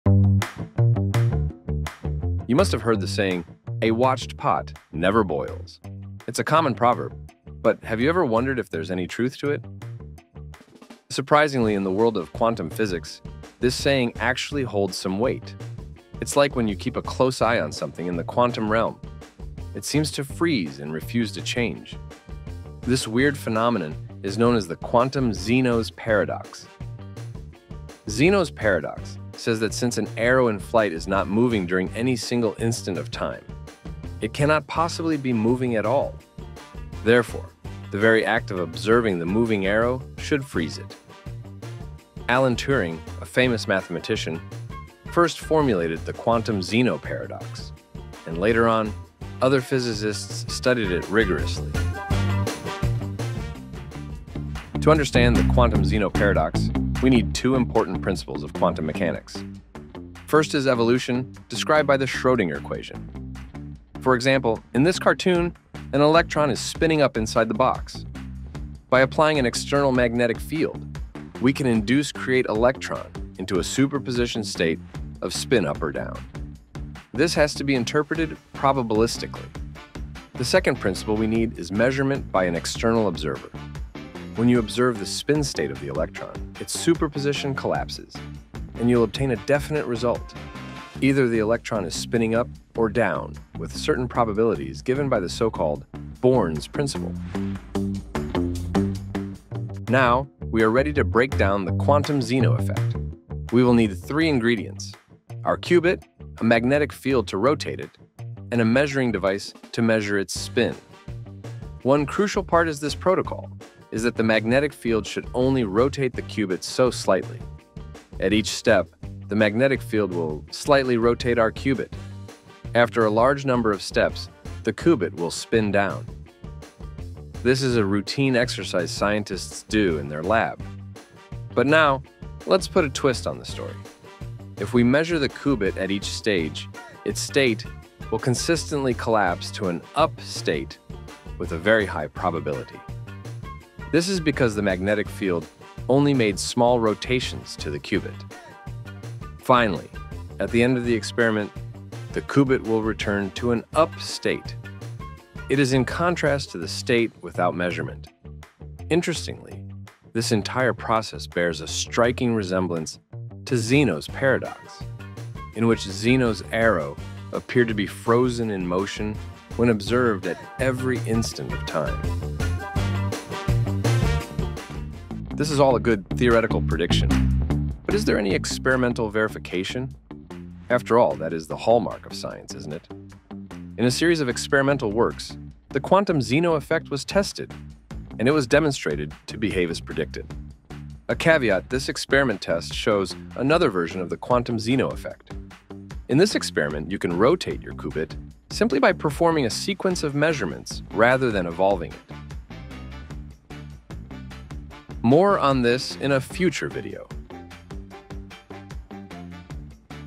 Quantum Zeno Effect in 3 minutes
Quantum Zeno Effect explained in simple terms

Measurement in quantum mechanics causes the system to collapse. If you frequenty observe i.e. perform measurement, you can essentially freeze the quantum system. This phenemenon is called Quantum Zeno effect, first discovered by Mathematician Alan Turing.